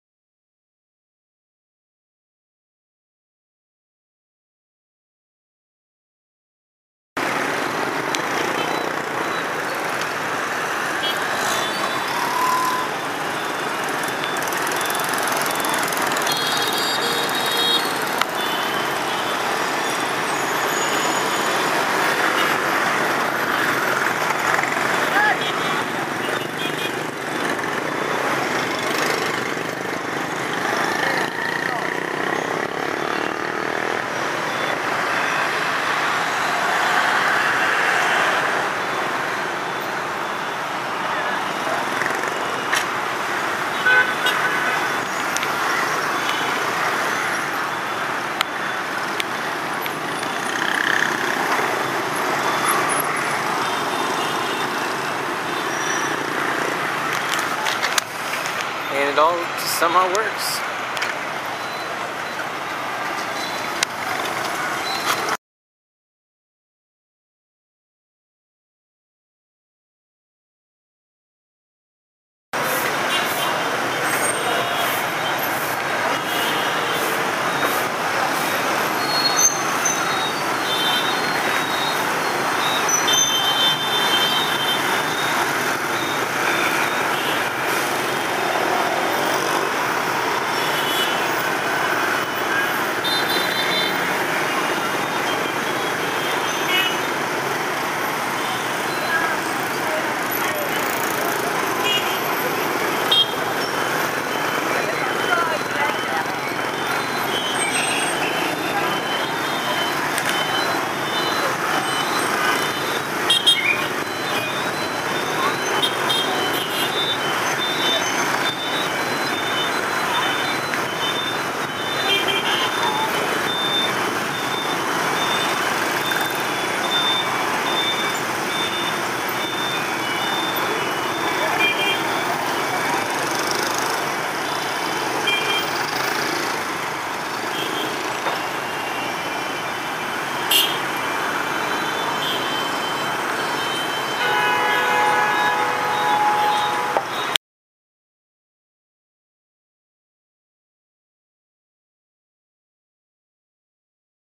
Crazy Traffic near Bui Vien, Ho Chi Minh City Vietnam 2
The guy on the motor bike with a tree talking on the phone is awesome.  Crossing the street was a huge thrill, with bikes and cars wizzing around you on all sides.